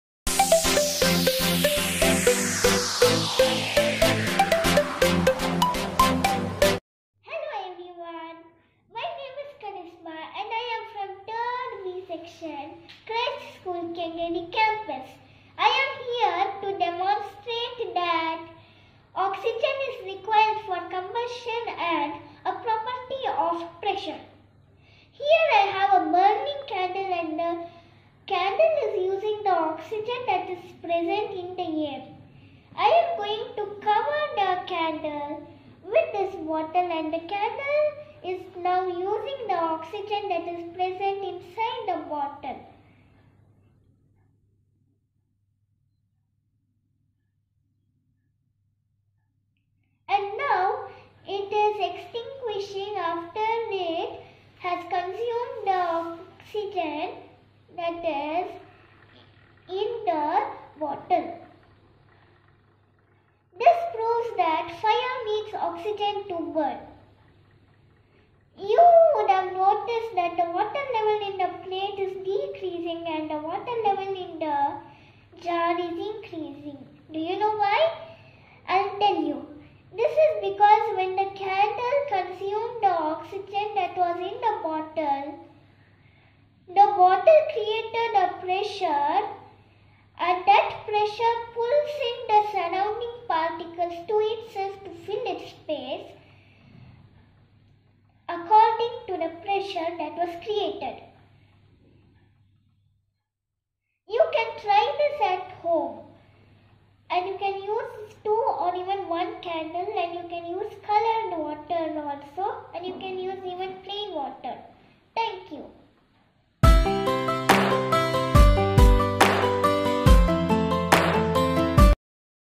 Combustion | 3B | Christ School Kengeri Campus
Student name - Karisma Santhosh Kumar
Grade -  3B
Subject -  Science
Topic - Combustion

Looking forward for all of your support.

Christ School Kengeri Campus
K.Gollahalli, Kumbalgodu Bengaluru -560026
Face book: Christ School Kengeri Campus